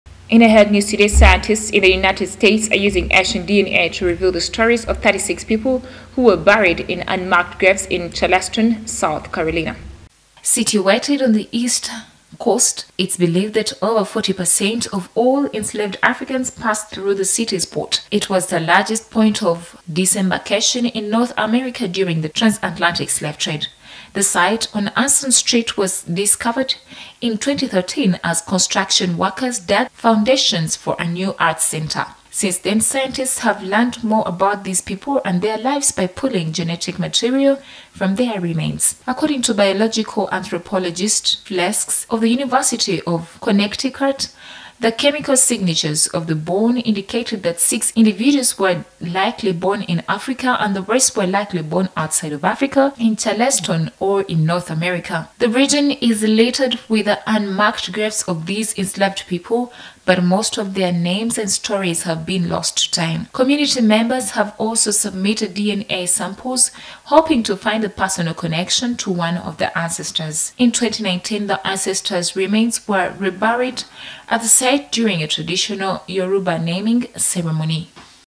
AFRICA : ORIGIN OF HUMANITY; Scientists use DNA to uncover story of Charleston slaves
Scientists in the United States are using ancient DNA to reveal the stories of 36 people who were buried in unmarked graves in Charleston, South Carolina, in the late 1700s.